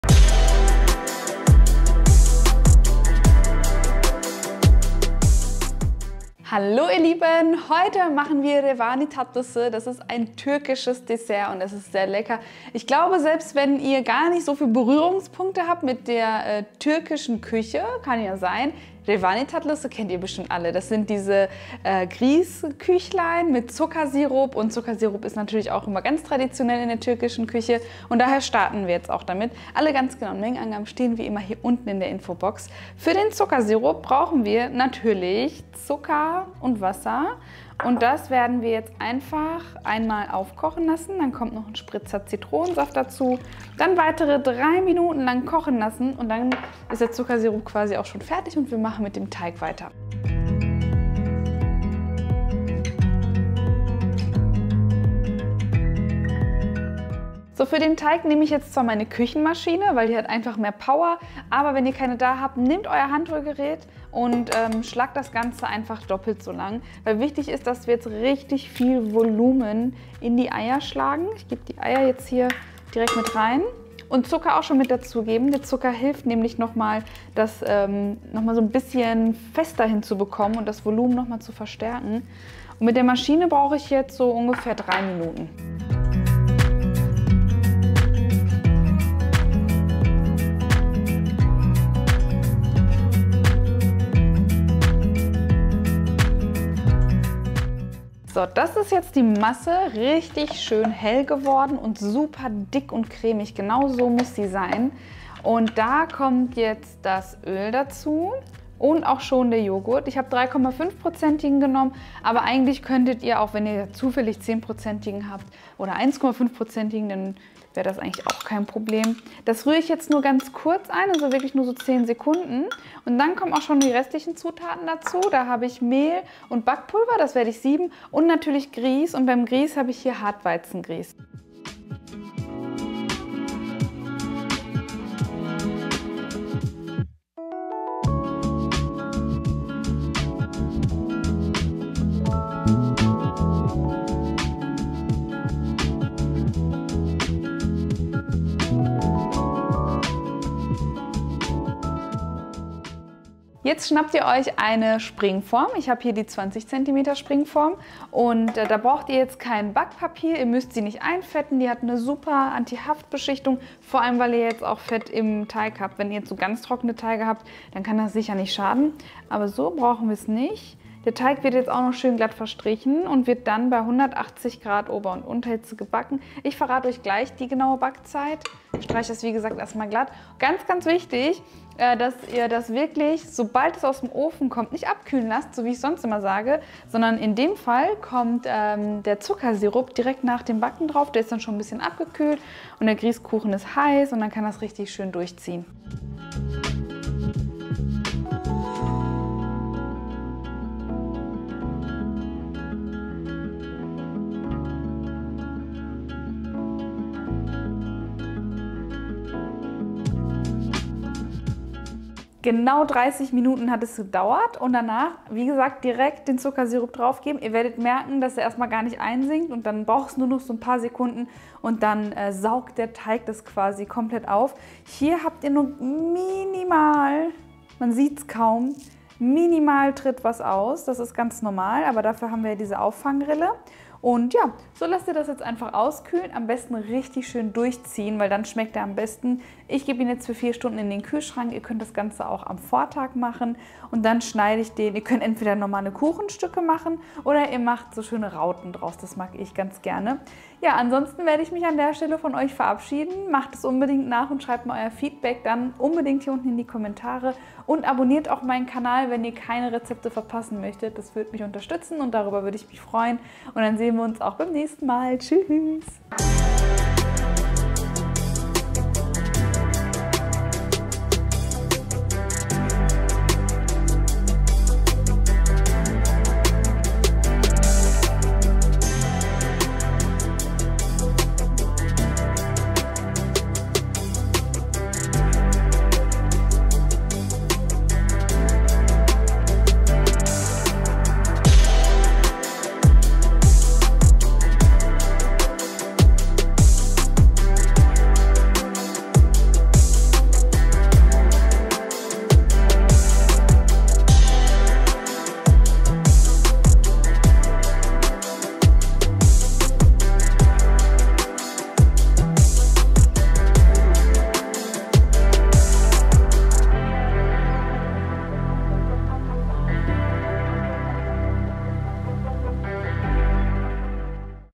der einfachste und saftigste Kuchen - Revani tatlısı: Grießkuchen Schnitten
Diese leckeren Schnitten gehen unfassbar einfach und bleiben aufgrund des Zuckersirups auch sehr lange frisch und saftig. Das perfekte Dessert für wenn man wenig Zeit hat oder einfach auch keine Lust auf etwas aufwendiges hat und trotzdem seine Gäste begeistern möchte.
Wir waren begeistert 😍

Viel Spaß beim nachmachen und lasst dem Video doch gerne einen Kommentar da was wir als nächstes machen sollen ❤️

Tragt euch für meinen kostenlosen Newsletter ein, um Aktionen und Sneak Peaks nicht zu verpassen

▶️ Schwierigkeitsgrad: Einfach
▶️ Zubereitungsdauer: 45 Min.
▶️ Portionen: 14
▶️ Durchmesser: 20 cm

▶️  Z U T A T E N

2 Wassergl. Zucker
2 Wassergl. Wasser
1 TL Zitronensaft

Für den Teig:
2 Eier
65 gZucker
150 g Joghurt 3,5 %
100 g Öl
200 g Hartweizengrieß
115 g Mehl
10 g Backpulver

▶️ Nährwerte pro Portion:
253 kcal / 1059 kJ
39 g Kohlenhydrate
5 g Eiweiß
8 g Fett

0:00 Intro
0:06 Begrüßung
0:32 Zuckersirup zubereiten
0:57 Teig zubereiten
2:33 Teig in Form einfüllen und backen
3:23 Kuchen nach dem backen mit Zuckersirup übergießen
3:53 auskühlen lassen, schneiden und dekorieren
4:51 Endergebnis
5:13 Outro